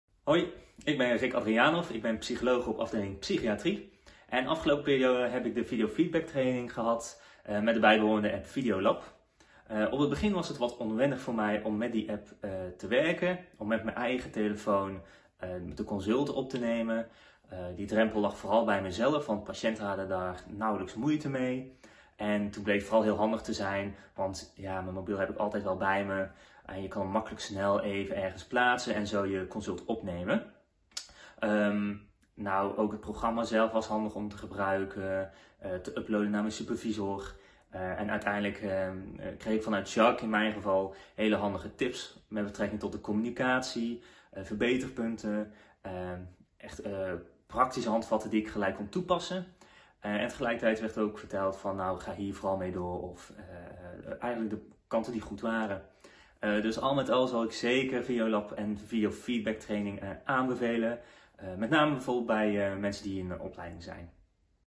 Video Feedback Training Experience
In this Video a psychologist shares his experience with Video Feedback training using Videolab.